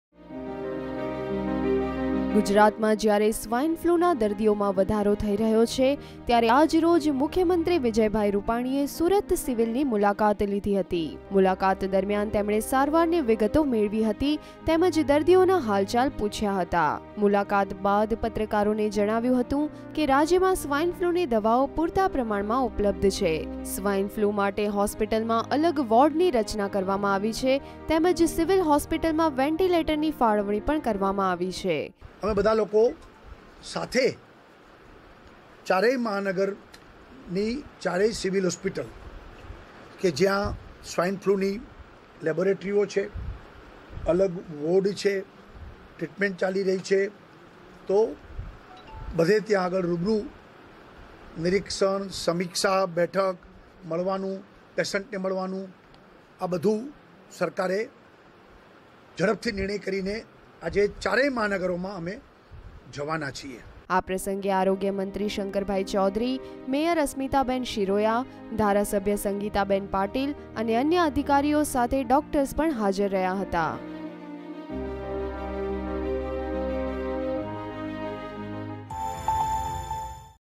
Gujarat CM Vijay Rupani visited Surat Civil Hospital to take stock of swine flu treatment
With increasing number of swine flu cases reported across the State, Chief Minister Vijay Rupani personally visited Surat Civil Hospital to review the preparedness and treatment been provided to the patients.